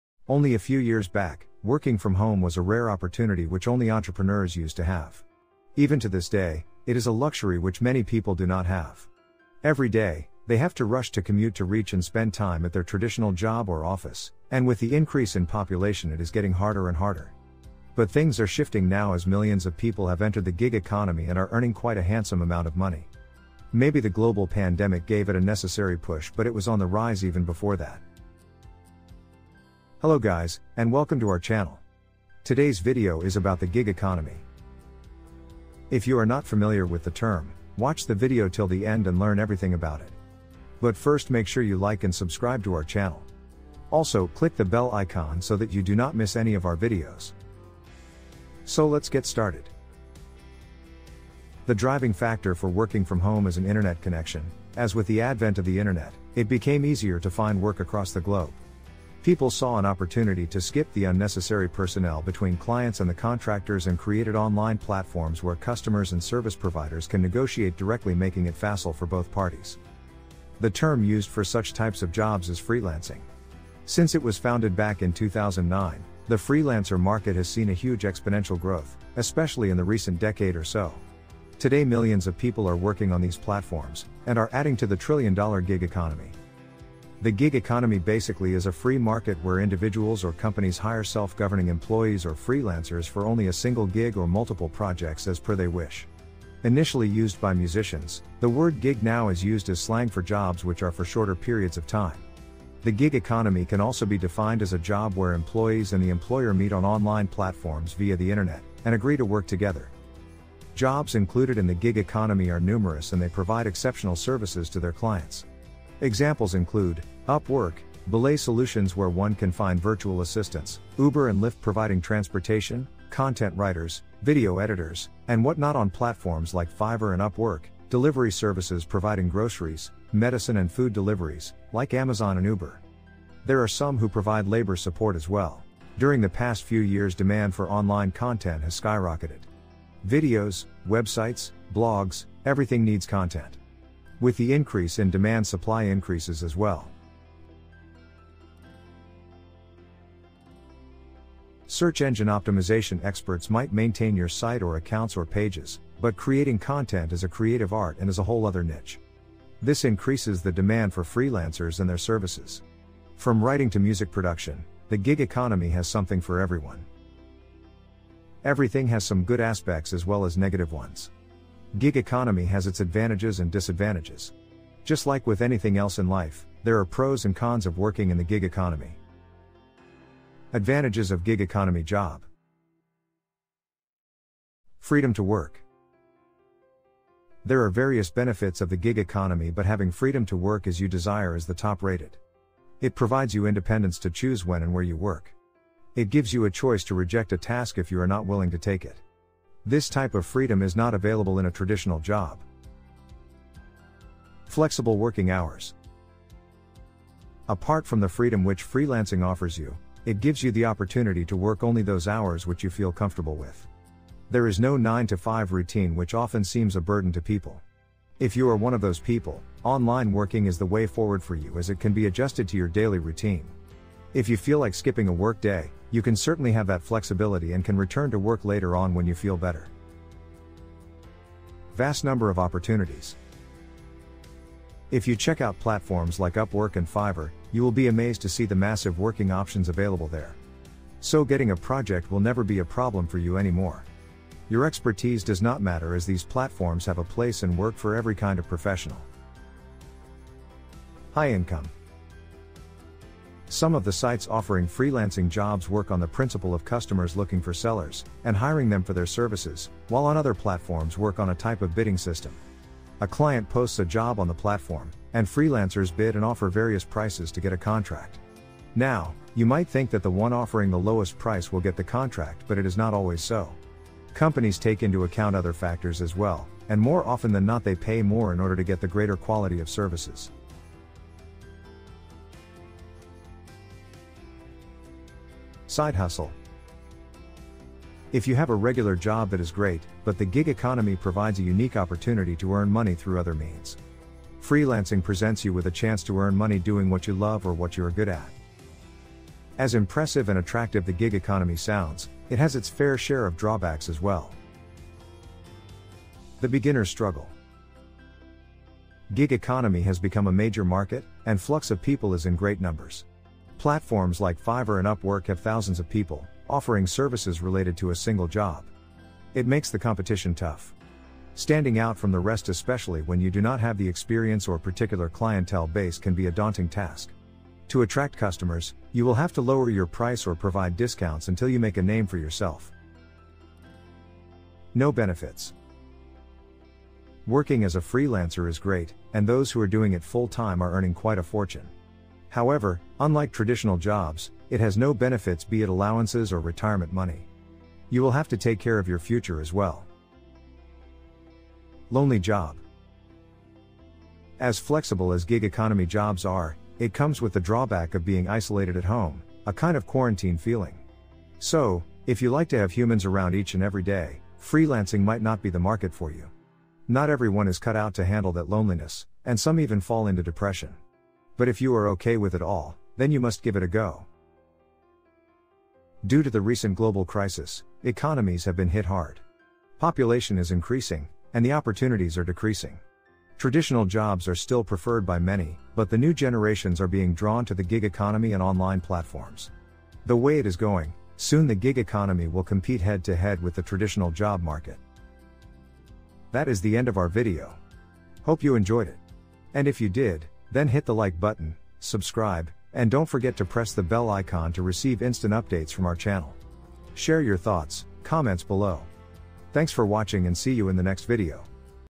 The Gig Economy Explained
Only a few years back, working from home was a rare opportunity that only entrepreneurs used to have. Even to this day, it is a luxury that many people do not have. Every day, they have to rush to commute to reach and spend time at their traditional job or office, and with the increase in population, it is getting harder and harder. But things are shifting now as millions of people have entered the gig economy and are earning quite a handsome amount of money. Maybe the global pandemic gave it a necessary push but it was on the rise even before that.
The driving factor for working from home is an internet connection as with the advent of the internet, it became easier to find work across the globe. People saw an opportunity to skip the unnecessary personnel between clients and the contractors and created online platforms where customers and service providers can negotiate directly making it facile for both parties.
The term used for such types of jobs is freelancing. Since it was founded back in 2009 the freelancer market has seen a huge exponential growth, especially in the recent decade or so. Today millions of people are working on these platforms and are adding to the trillion-dollar gig economy.

Today’s video is about the gig economy. If you are not familiar with the term, watch the video till the end and learn everything about it.